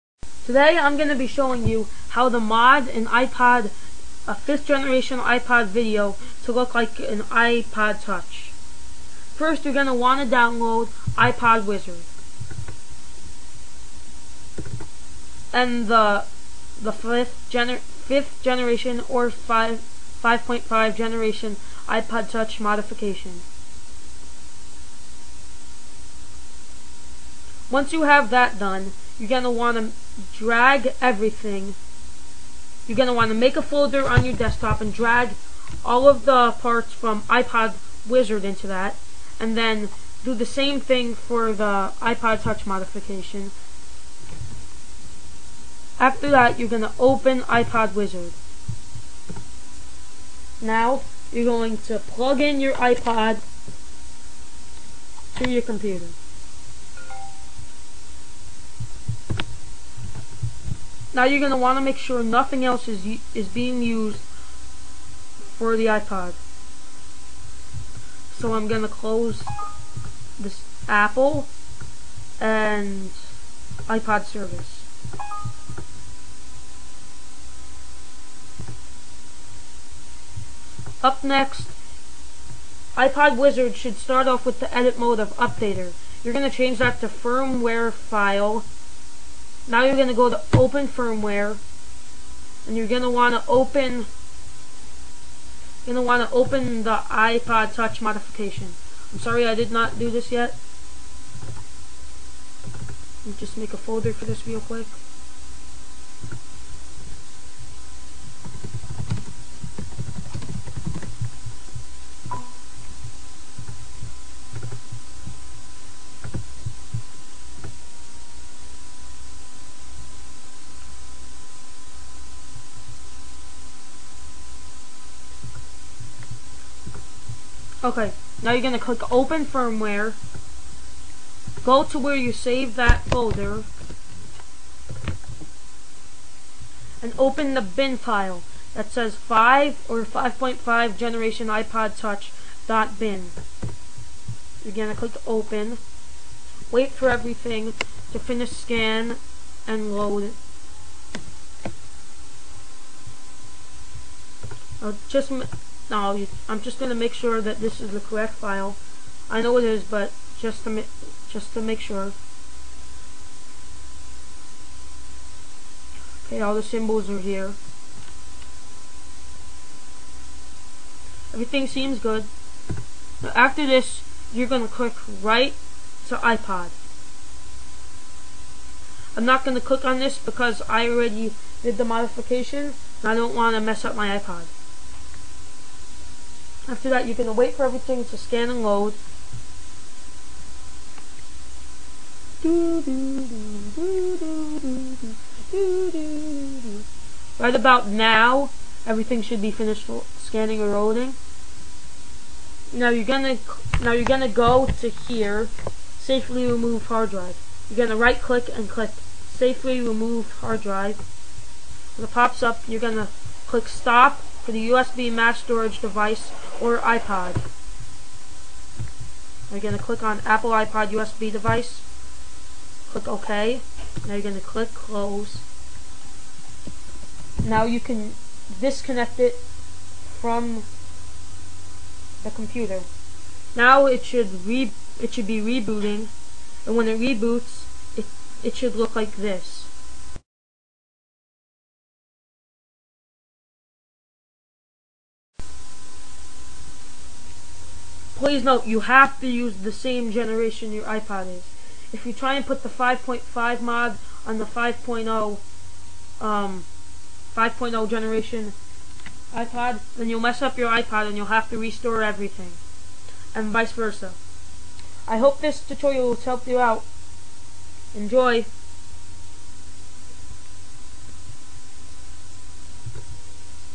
[Tutorial] Ipod Video To Ipod Touch
Here is a tutorial to turn a normal looking ipod video (5 generation or 5.5 generation) to look like Ipod Touch, but you can't touch on the screen, you'll still have to use the pinwheel, though.